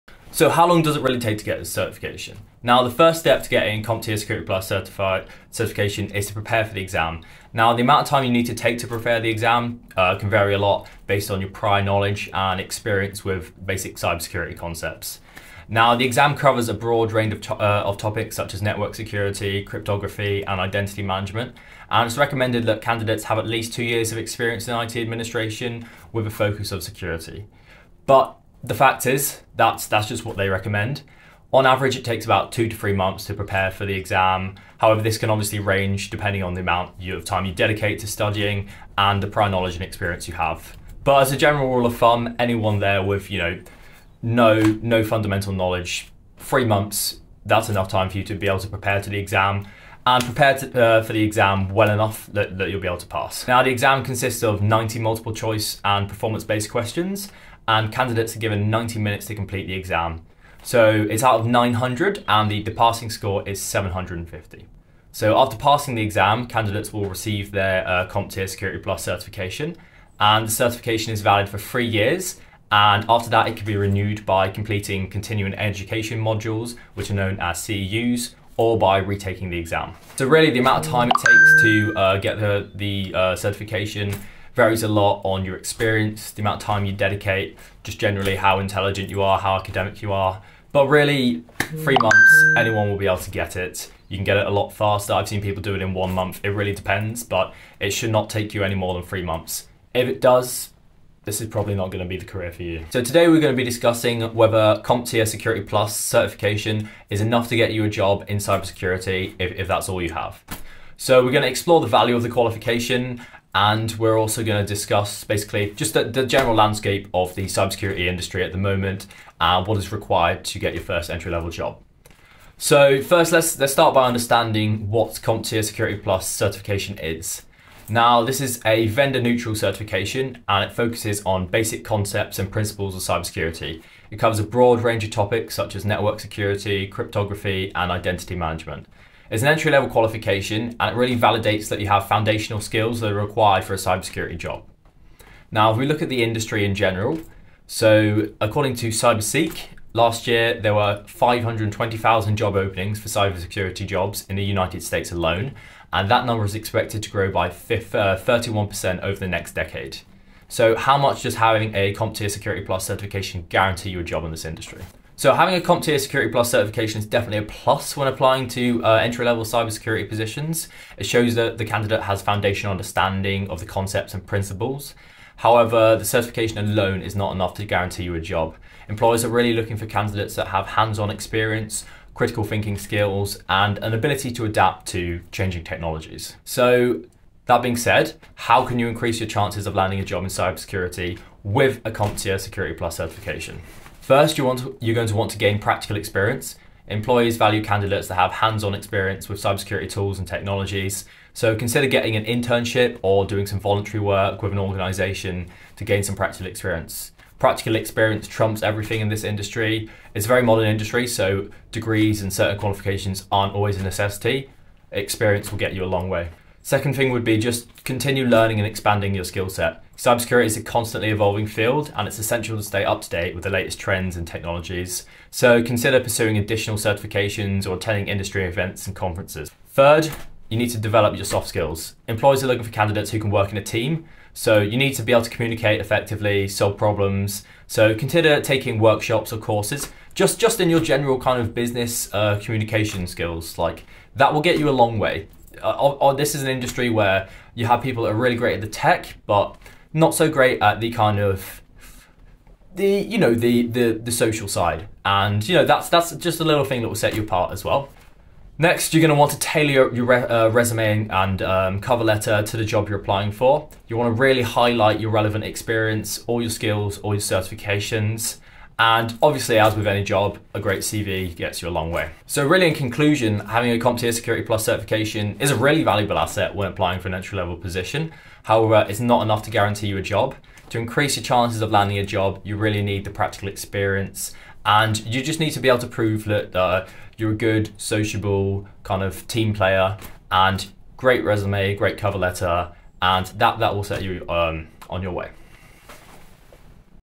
How Long does CompTIA + Take? | Is It Enough To Get a Job?
How long does it take to get CompTIA Security+ certification, and is it enough to get a job?

Firstly, let's look at the time it takes to get certified. The actual time it takes to get certified depends on various factors, such as the individual's experience, study habits, and the training program used. On average, it can take 2-3 months of dedicated study to prepare for the CompTIA Security+ certification exam. However, if you have experience in the field or a background in IT, it may take less time.

The certification exam itself consists of 90 questions, which need to be completed within 90 minutes. The exam questions cover a range of topics related to information security, such as threats, vulnerabilities, network security, cryptography, and risk management. To pass the exam, candidates need to score at least 750 out of 900.

Now, let's address the second part of the question: is the CompTIA Security+ certification enough to get a job? While the certification can certainly help you stand out from other candidates, it is not a guarantee of employment. Employers generally look for a combination of education, experience, and certification when hiring cybersecurity professionals.

The CompTIA Security+ certification is a good starting point for those who are just entering the cybersecurity field or looking to advance their careers. However, it is important to note that the certification alone may not be enough to land a job. It is important to gain practical experience and skills, either through internships, volunteer work, or entry-level positions.

Additionally, there are several other certifications in the cybersecurity field that may enhance your employability, such as Certified Information Systems Security Professional (CISSP), Certified Ethical Hacker (CEH), and Certified Information Security Manager (CISM).

In conclusion, the time it takes to get CompTIA Security+ certification varies depending on the individual's experience and study habits. On average, it takes 2-3 months of dedicated study. While the certification can certainly help you stand out from other candidates, it is not a guarantee of employment. It is important to gain practical experience and skills, in addition to the certification, to increase your chances of landing a cybersecurity job.

I appreciate everyone that watches ;)